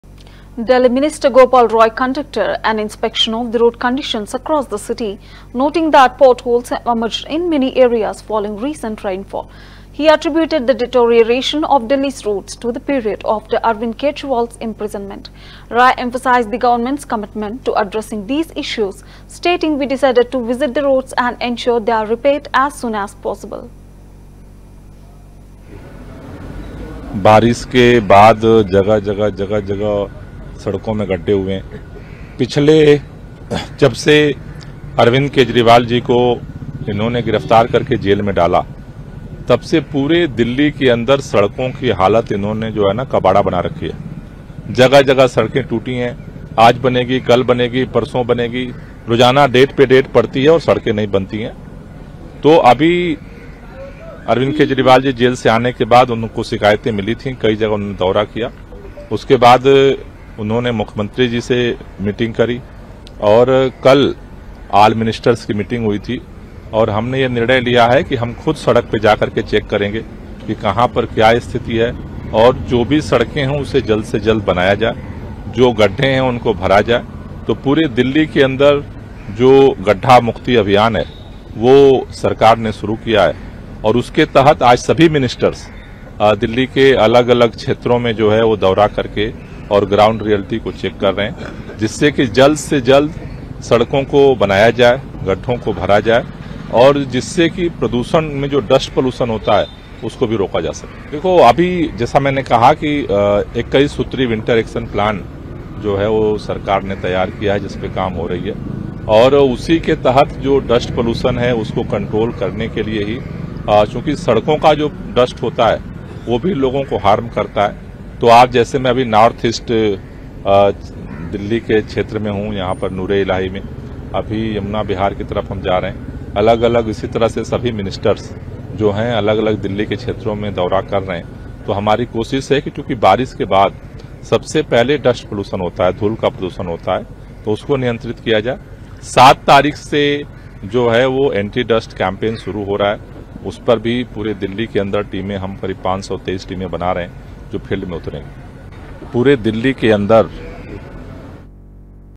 Gopal Rai Inspection Delhi Road: Delhi roads will be 'pothole free' as soon as possible | AAP | CVR
Gopal Rai Inspection Delhi Road: Delhi roads will be 'pothole free' as soon as possible - Gopal Rai | AAP | CVR ENGLISH